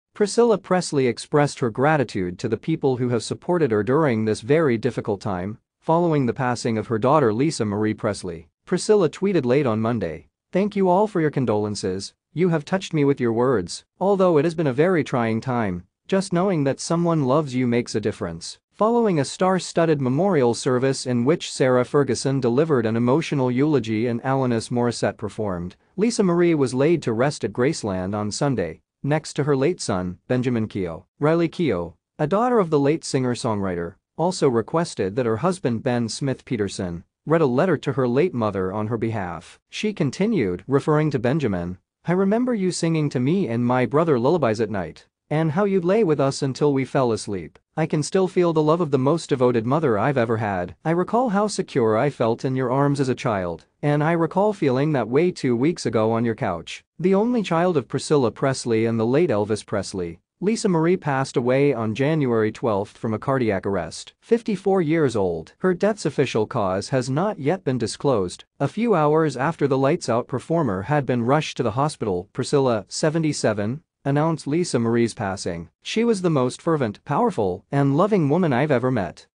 After Lisa Marie's funeral, Priscilla Presley describes it as a "very difficult time."
Priscilla Presley expressed her gratitude to the people who have supported her during this "very difficult time" following the passing of her daughter Lisa Marie Presley.
Priscilla tweeted late on Monday, "Thank you all for your condolences, you have touched me with your words.